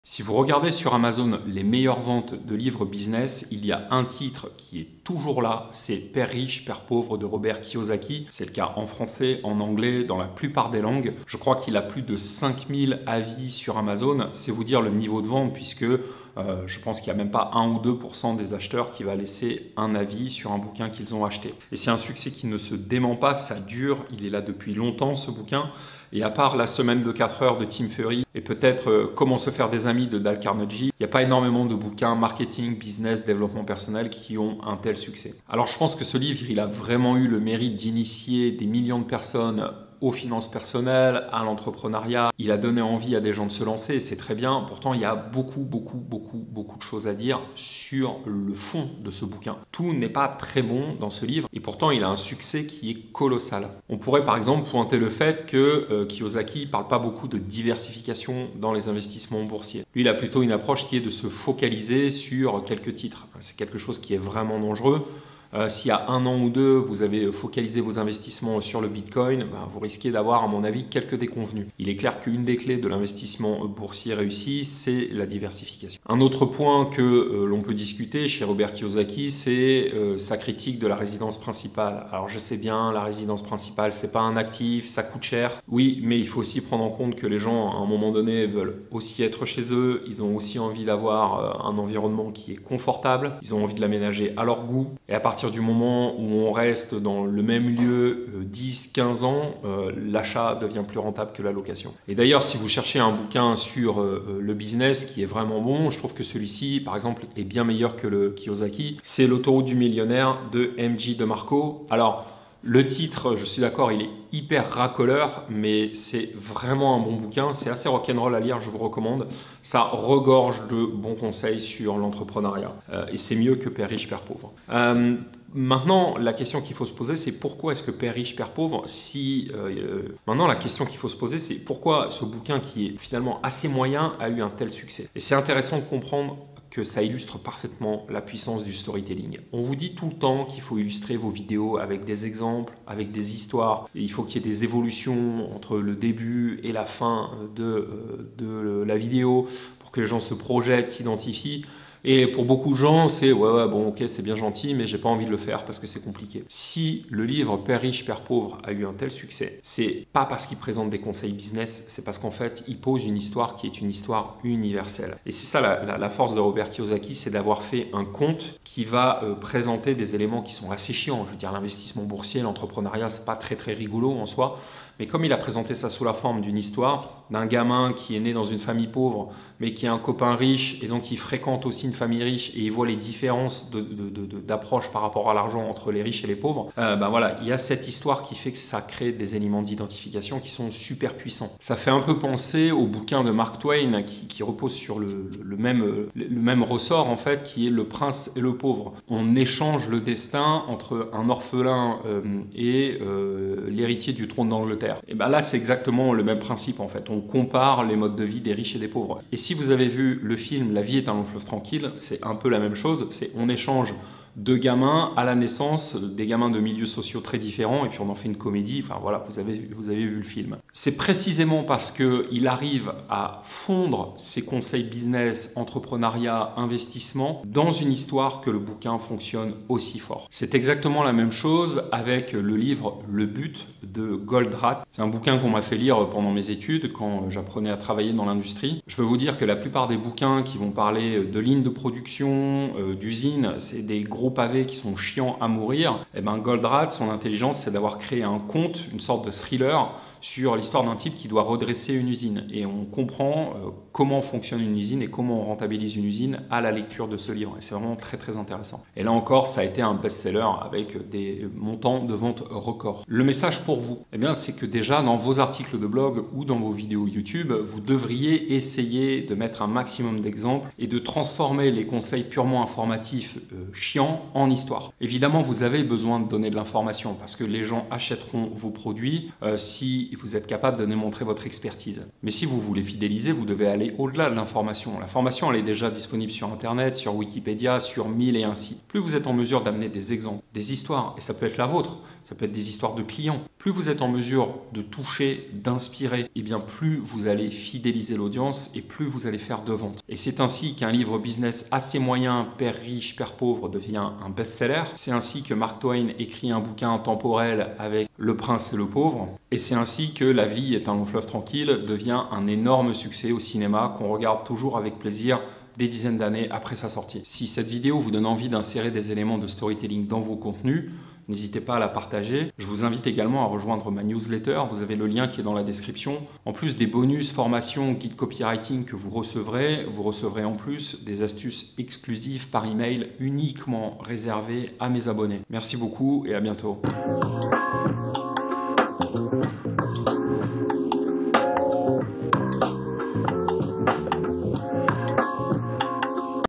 Pourquoi "Père riche, père pauvre" de Robert Kyosaki est un succès qui dure
Téléchargez votre formation trafic et votre guide copywriting sur les titres, c'est offert

Si vous regardez le rayon des lives business et marketing, le succès de "Père riche, père pauvre" ne se dément pas. Mais pourquoi, alors que le livre est assez moyen, se vend-il autant?